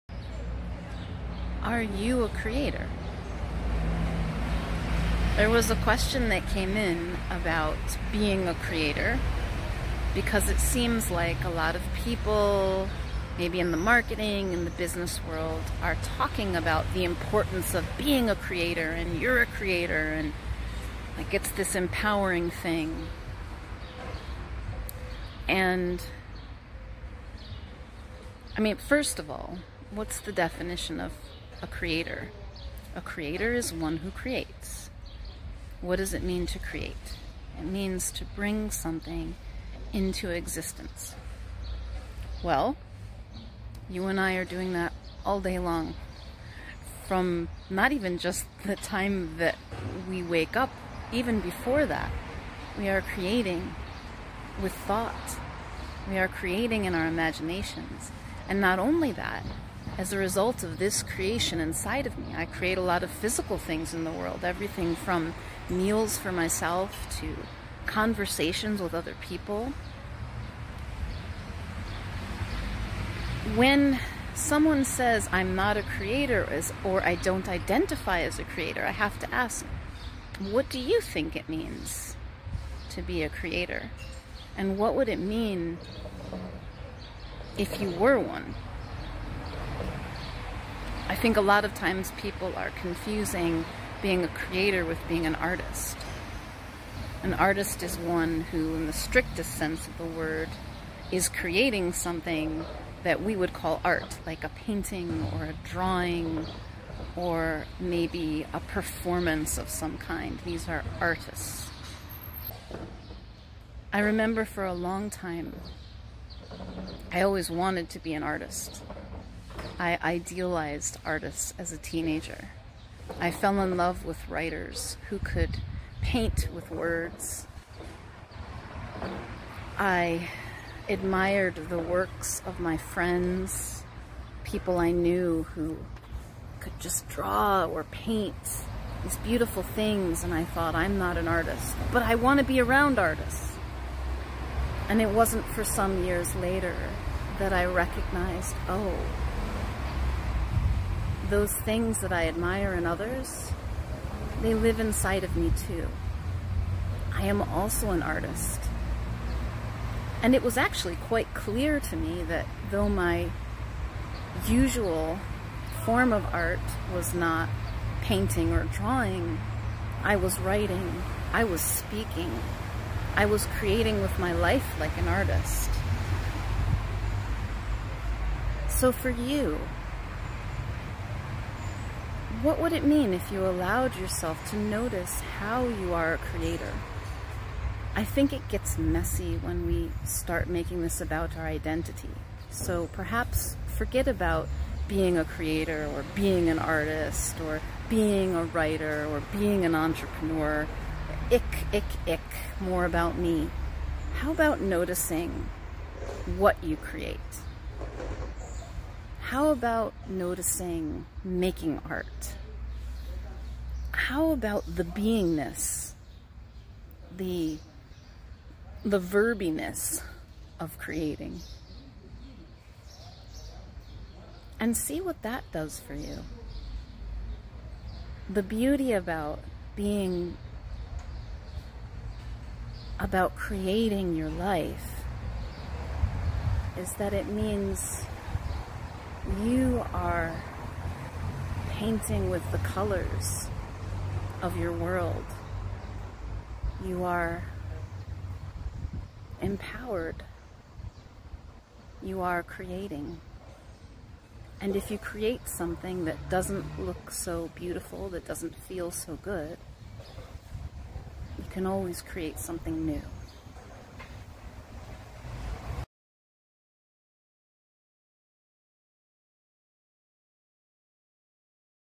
How to Know You're a Creator (Even If You're Not Sure)
What does it mean to be a "creator?" See through the creator hype to the truth of what it means to create your life and the empowerment it brings.